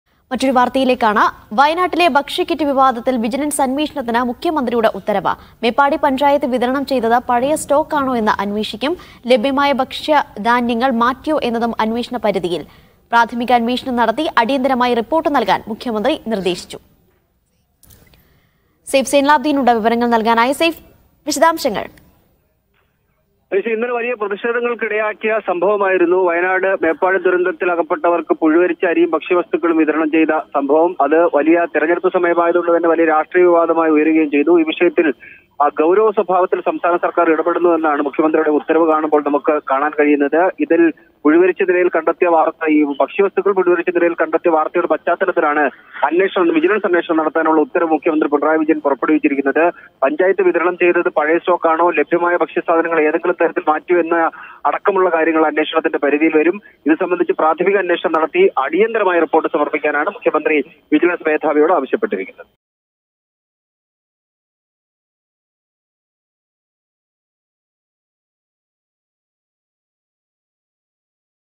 വയനാട്ടിലെ ഭക്ഷ്യക്കിറ്റ് വിവാദം; വിജിലന്‍സ് അന്വേഷണത്തിന് ഉത്തരവ്
വയനാട്ടിലെ ഭക്ഷ്യക്കിറ്റ് വിവാദത്തില്‍ വിജിലന്‍സ് അന്വേഷണത്തിന് ഉത്തരവിട്ട് മുഖ്യമന്ത്രി | wayanad | vigilence enquiry |

Genre: News
Language: Malayalam